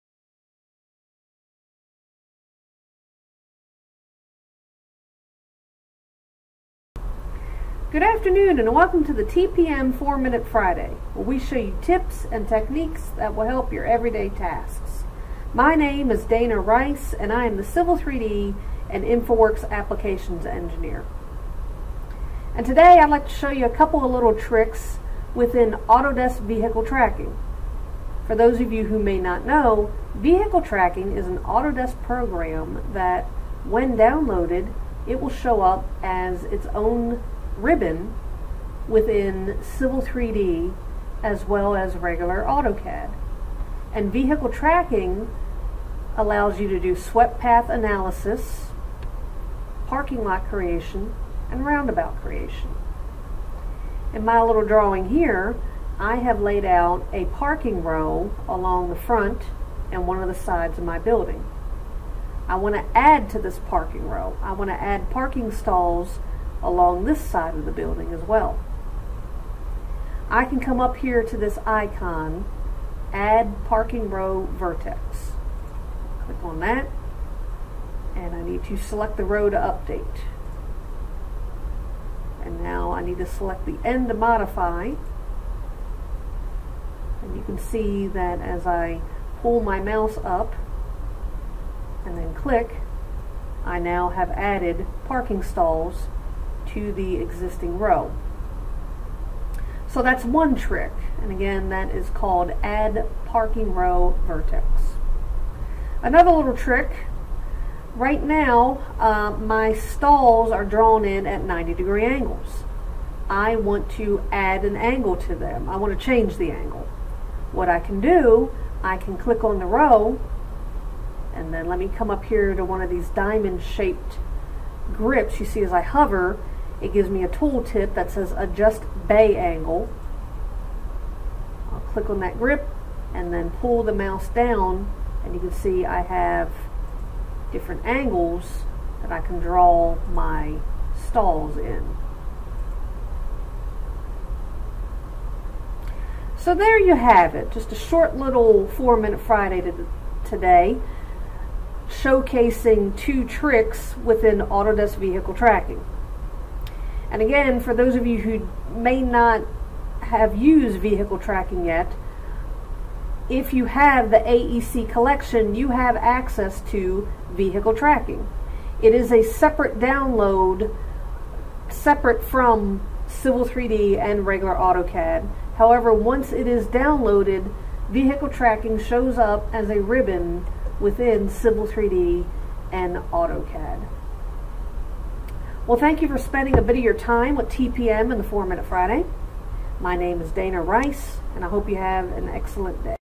Autodesk Vehicle Tracking Tips
Todays Four Minute Friday showcases a few tips and tricks to speed up your Autodesk Vehicle Tracking workflows.